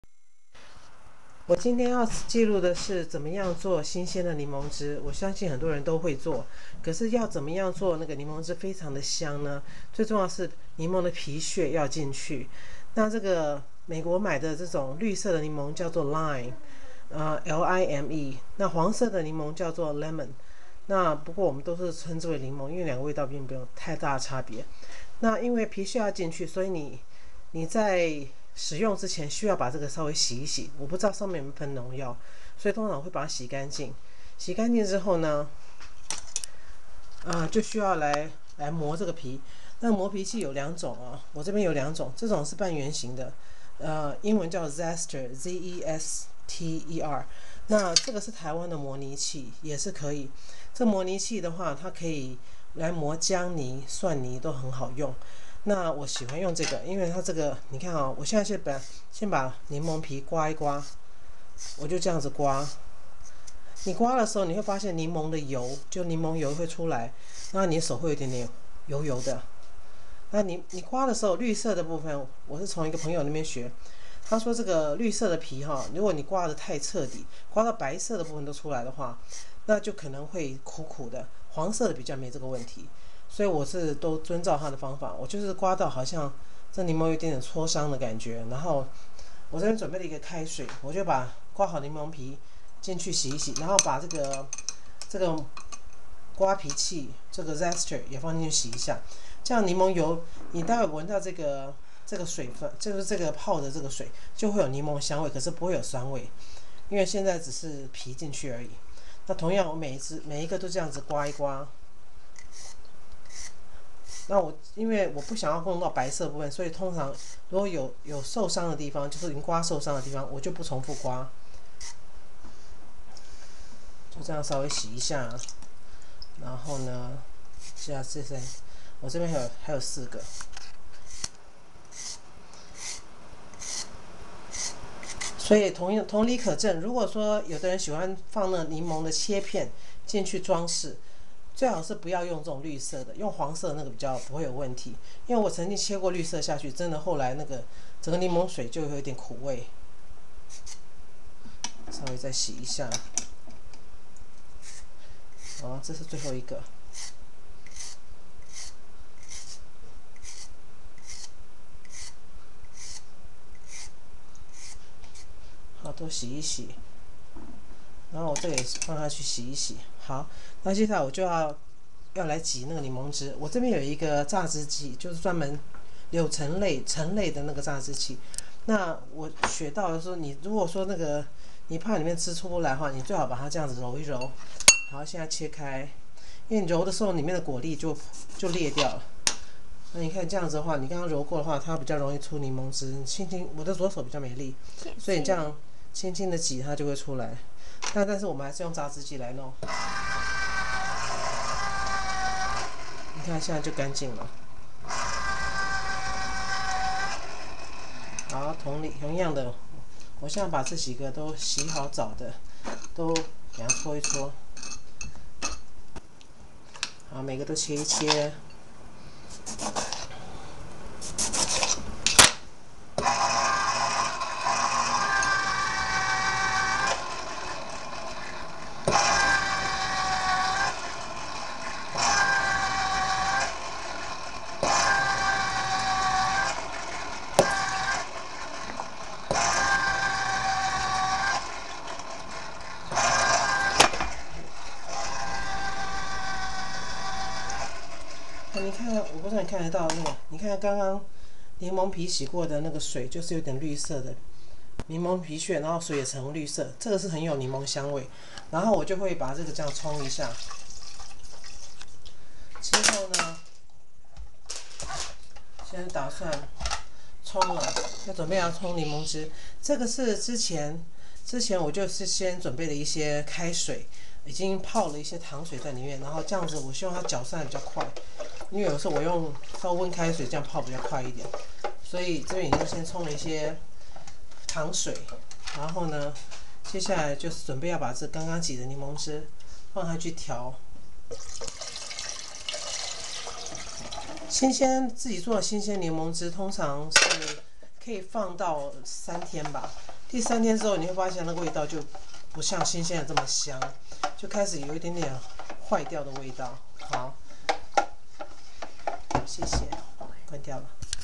榨出有香味的檸檬汁
使用於蘆薈汁調味。